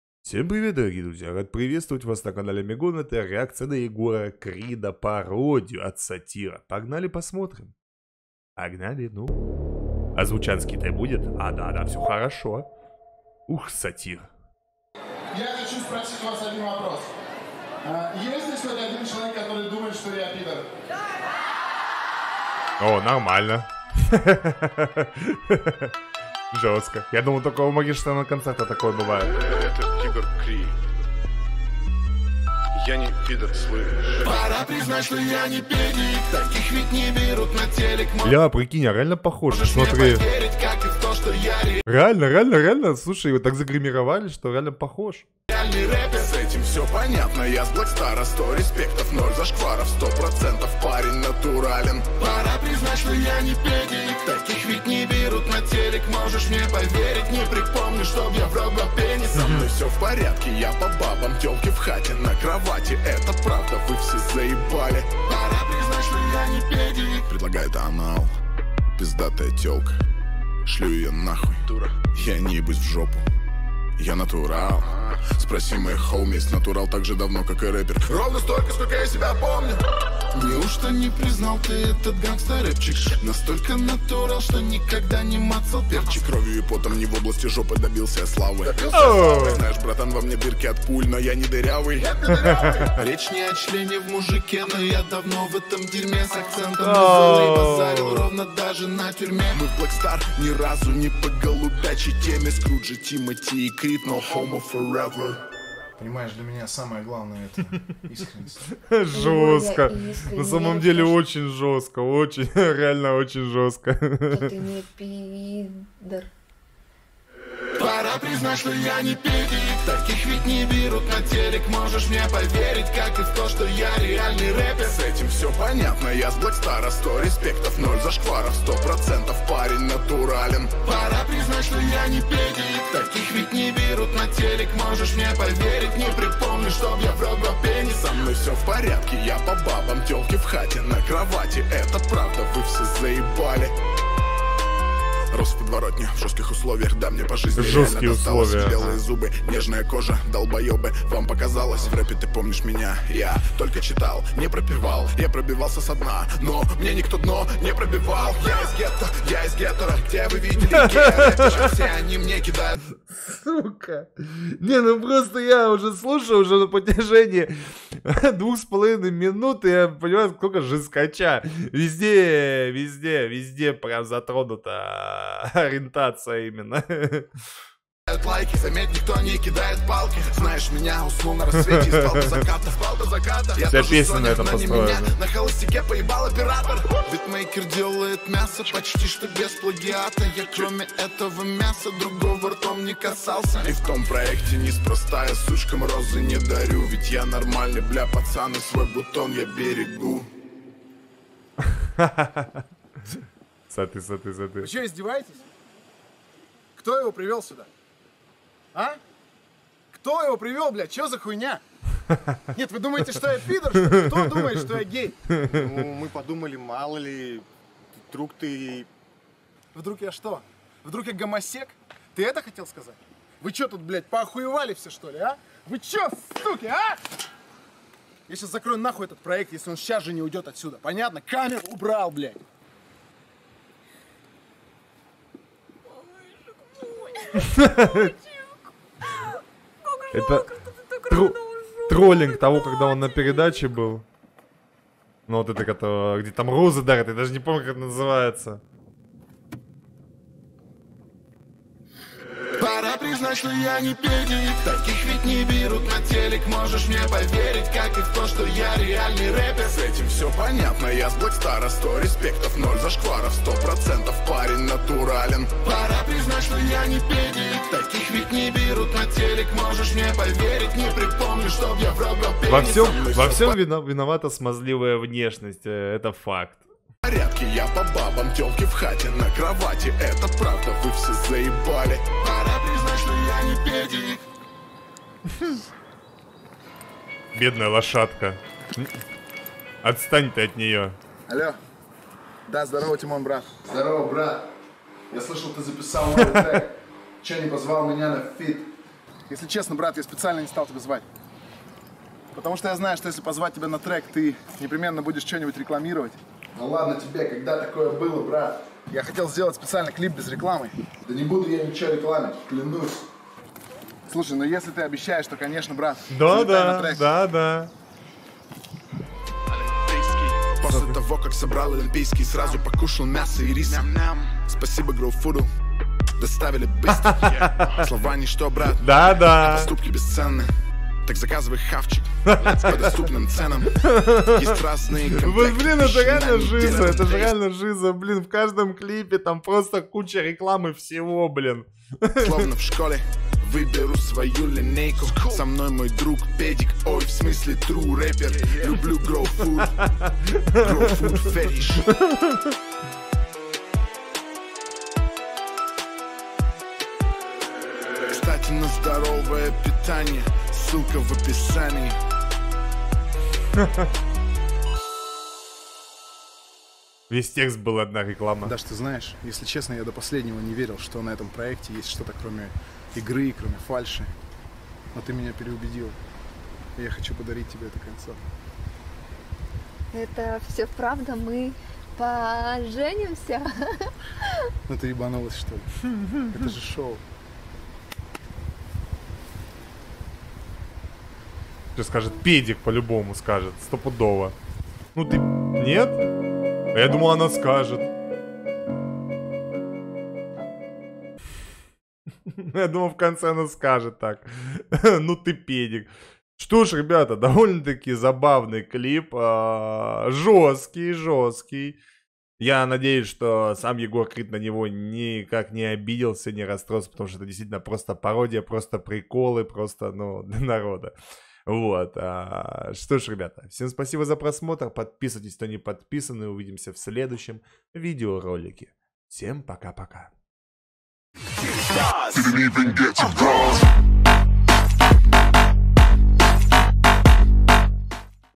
ЕГОР КРИД. ПАРОДИЯ #22 | 🤣РЕАКЦИЯ AMIGON
Реакция на интересное видео
🤝Автор оригинала : Satyr
🤝Название оригинала : ЕГОР КРИД. ПАРОДИЯ #22